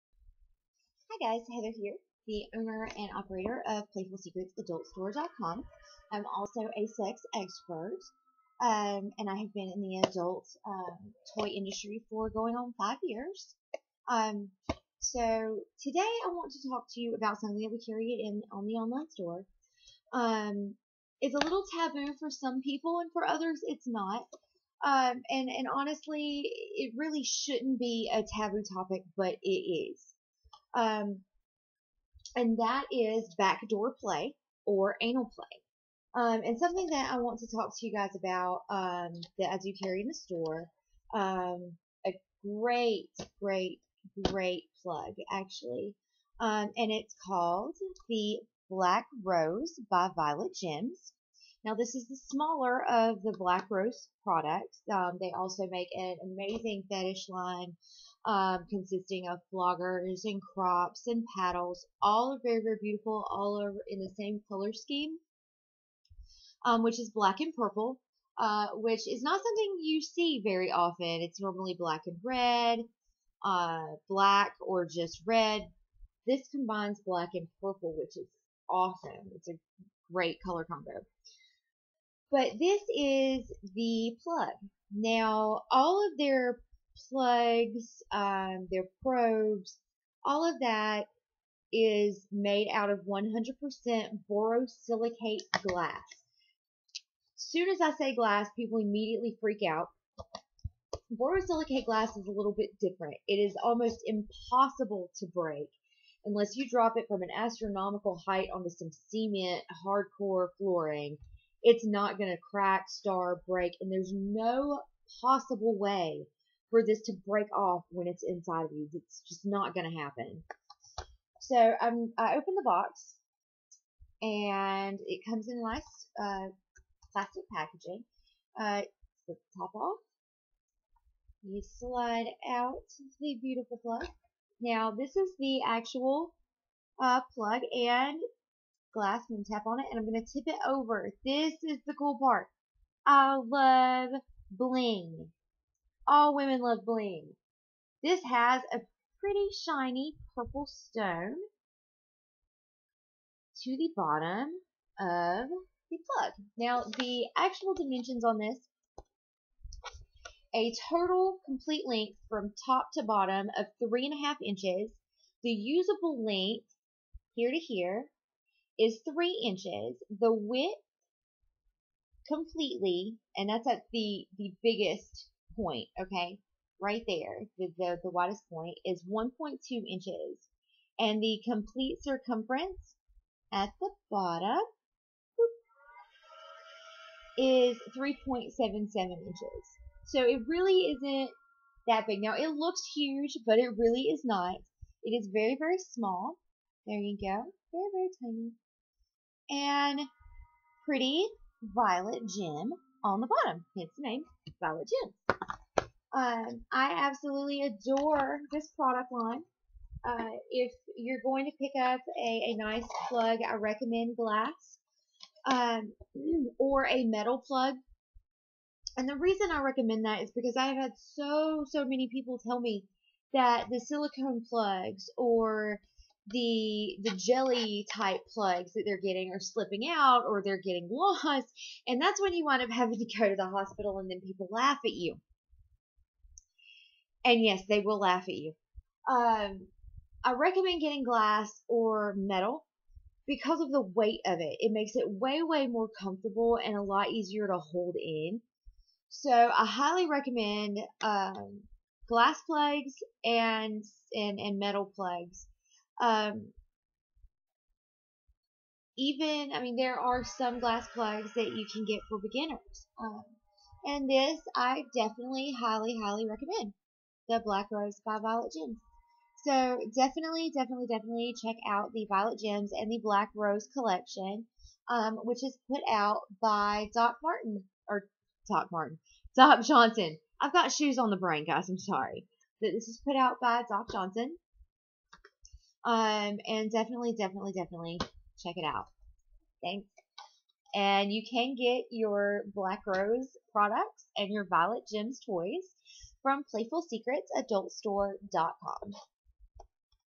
Violet Gems by Playful Secrets
In this video the owner of Playful Secrets discusses the Violet Gems and Black Rose Collection by Doc Johnson.